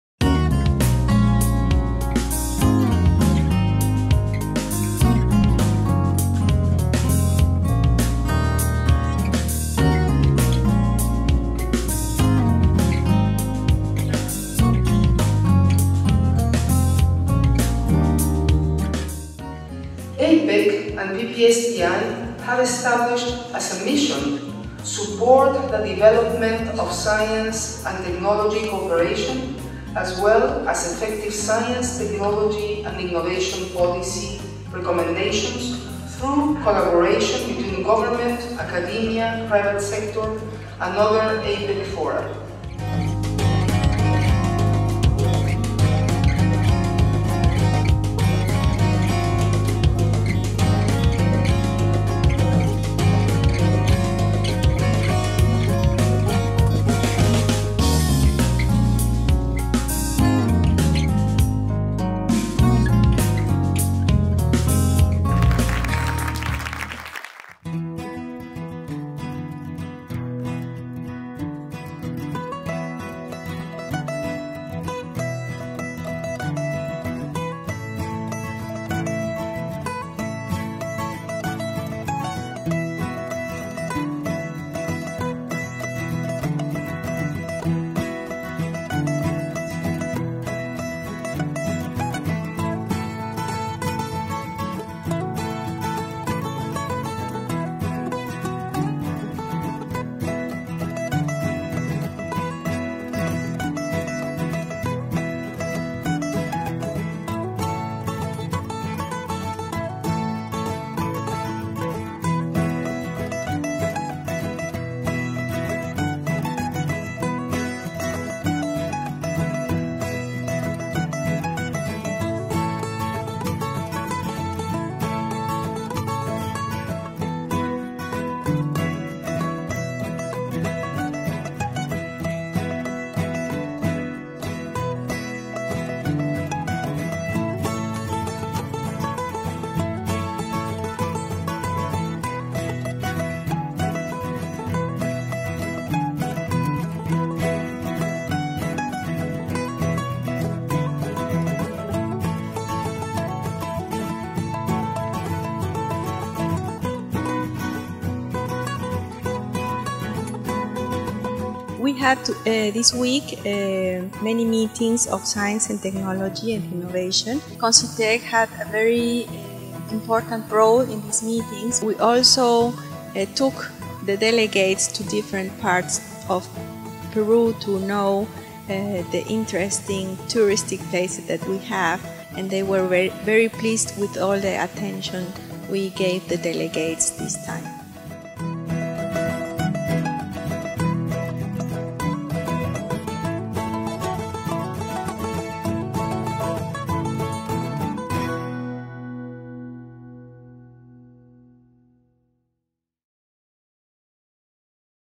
Grupo de trabajo de ciencia y tecnología del APEC se reunió en Lima
Como parte de las actividades preparatorias a la Cumbre de Líderes del Foro de Cooperación Económica Asia – Pacífico (APEC), se realizó la 8va reunión del “Grupo de Trabajo - Partenariado de Políticas de Ciencia, Tecnología e Innovación (PPSTI)”, en un evento desarrollado en el Centro de Convenciones de Lima, en el distrito de San Borja.

La reunión congregó a representantes vinculado a los temas de ciencia y tecnología de las 21 economías que integran el Foro de Cooperación Económica de Asia y Pacífico (APEC), que se realizó del 15 al 19 de agosto del 2016.